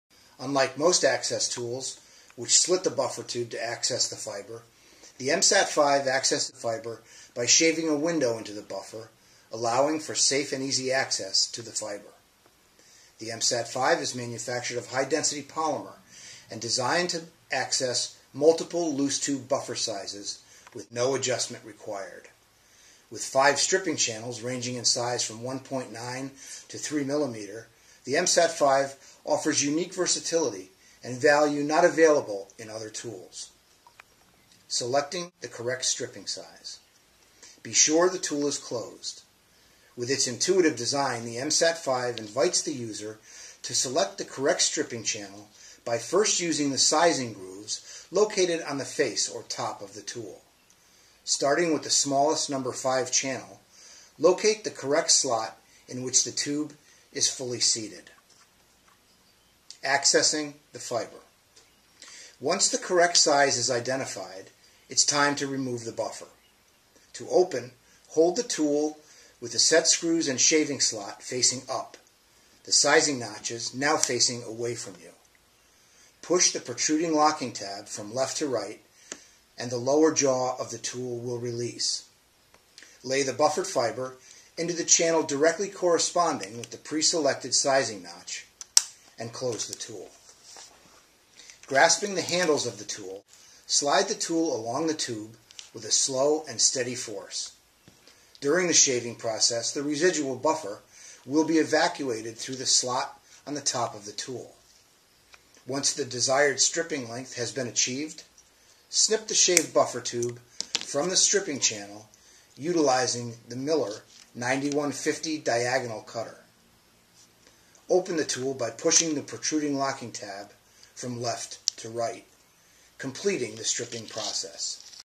How to strip buffer tube fibers with Miller's MSAT-5 buffer tube stripper?
The Miller Mid-Span Access Tool (MSAT) is used to quickly and easily access fibers in 1.9mm to 3mm buffer tubes in a mid-span location without damage to fiber.

No blade adjustment is necessary, as the factory precisely mounts the blades in the tube slitting channels, resulting in consistent and accurate slitting.

The tool design incorporates buffer tube size gauges on the tool body for ease of selecting proper tube slitting channel.